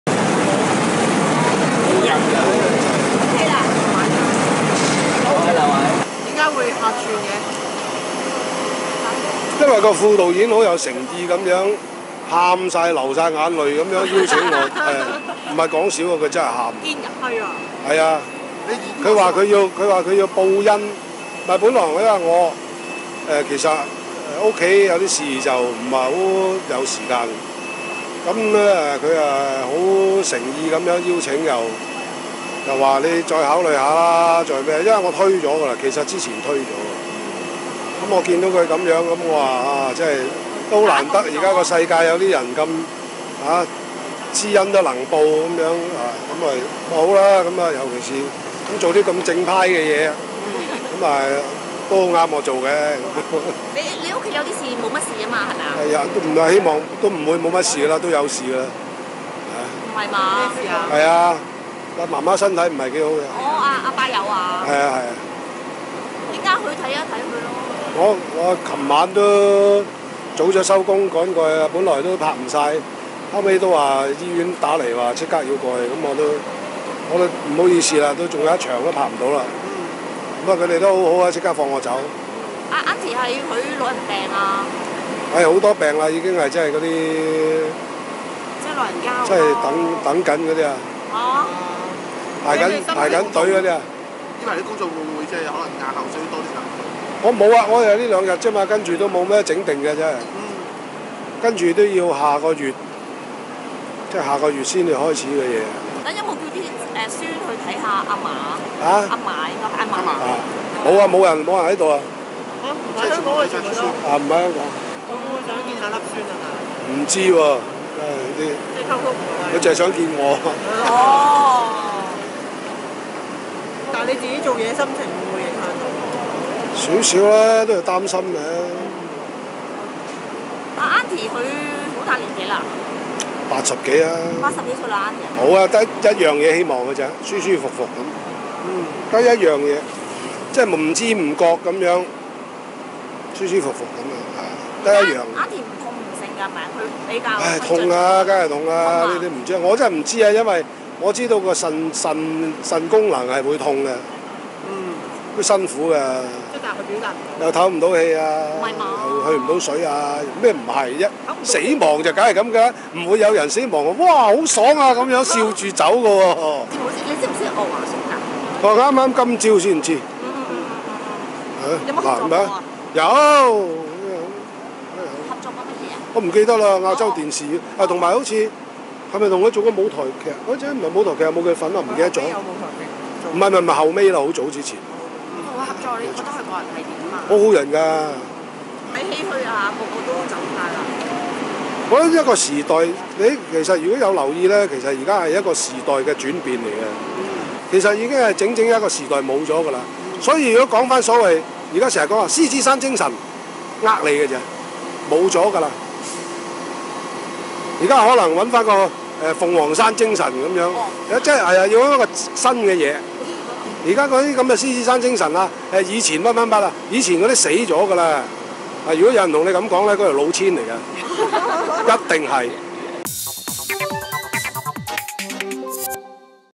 【祝福】媽媽身體抱恙留醫 秋生：情況不太好
黃秋生和張松枝今日穿上消防隊目戰衣為港台劇集《火速救兵IV》拍攝，他感慨說：「這兩日開工，媽媽身體不太好，昨晚最後一場戲都拍不到，因醫院打來要我趕過去，媽媽身體好多毛病，腎功能衰退、透不到氣，去不到水，情況不太好，都80幾歲，只是等緊時間。開工心情點都有少少影響，好擔心，我得一個希望，就是想她舒舒服服，不太辛苦。」問到會否安排妻子見媽媽？秋生說：「她沒有意識，孫仔全部不在香港，要返學讀書，她最想見的人是我！」